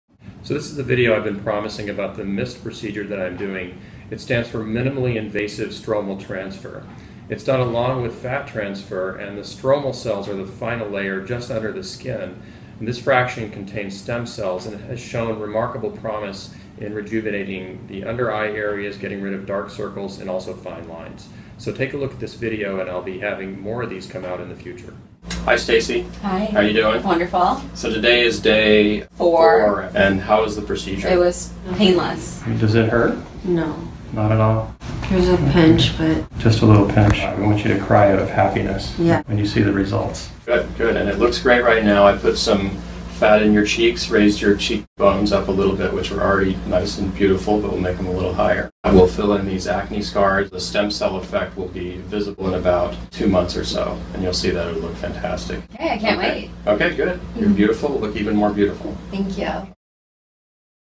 Dr. Marc Mani - MIST procedure
Dr. Marc Mani describes and performs facial regeneration with his new MIST procedure.  Micro fat is injected for volume when needed and stromal cells (including stem cells and other cells which nourish them) are placed into the dermis and skin itself.  This reconstructs the skin from within, treating fine lines, dark under eye circles, scars, and other problems from within - unlike lasers which destroy skin from outside.